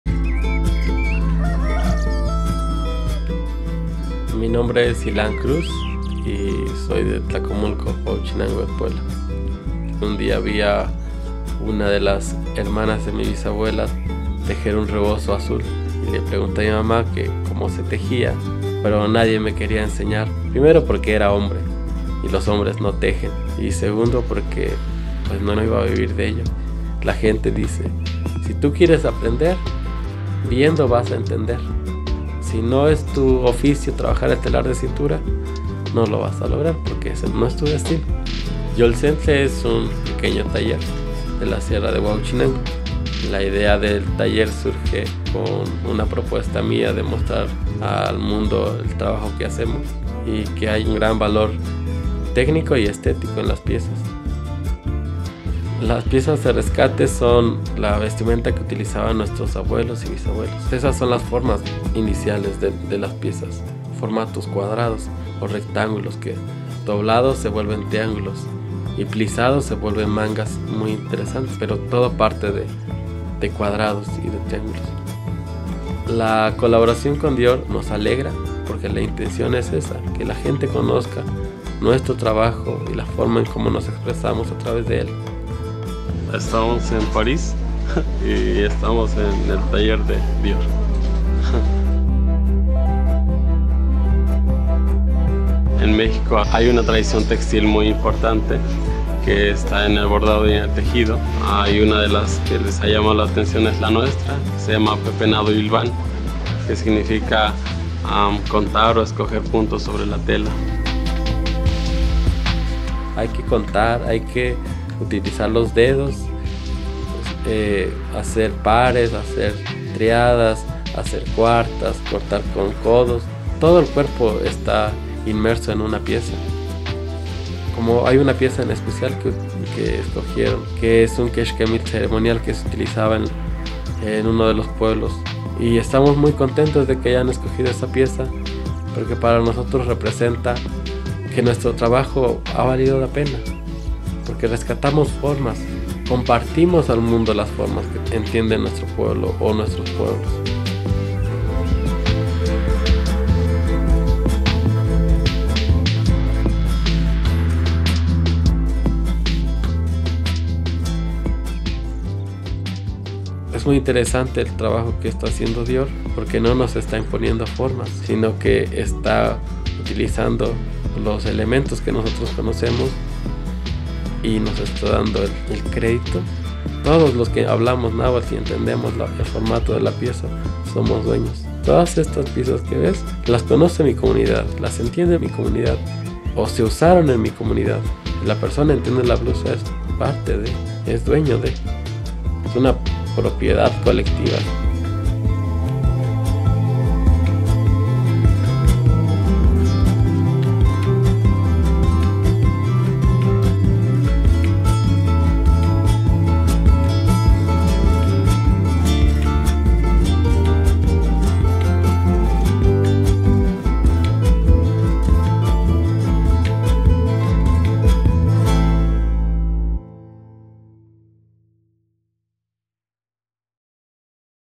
Dior Cruise 2024 Pueblan Savoir-Faire
Developing a passion for traditional Mexican textiles at a young age, learn how the weaver Hilán Cruz and the artisans of the Yolcentle Taller he co-founded in the state of Puebla collaborated with the House on the creation of several hand-crafted pieces for the Dior Cruise 2024 collection by Maria Grazia Chiuri, unveiled in the spectacular show in Mexico City.